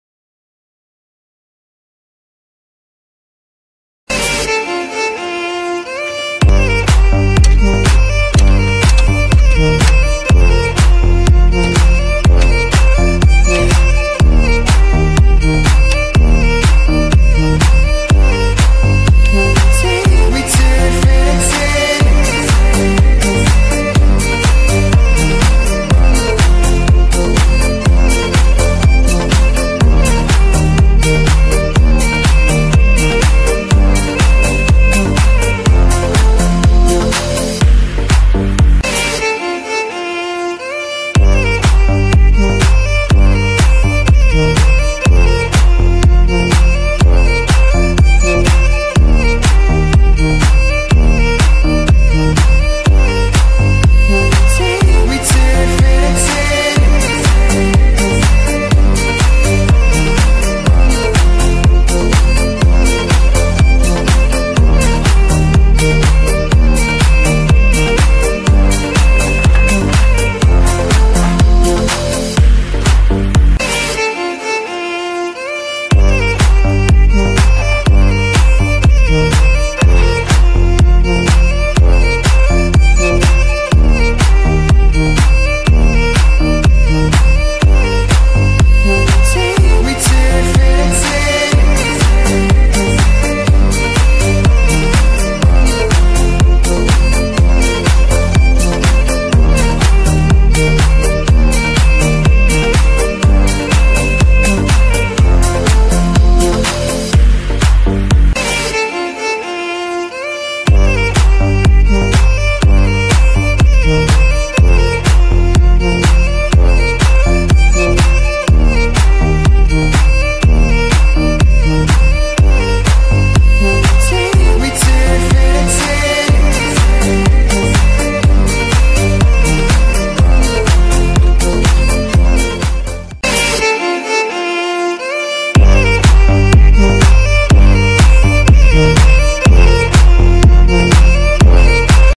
LONGYIE D7001 Dry inkjet minilab photo printing machine photo lab equipment
LONGYIE D7001 Dry inkjet minilab photo printing machine photo lab equipmnt PK fuji frontier DX100 fuji frontier DE100   EPSON D701
Print max 12inchx44inch
automatic loading paper, automatic cutting and automatic drying, continious  ink supply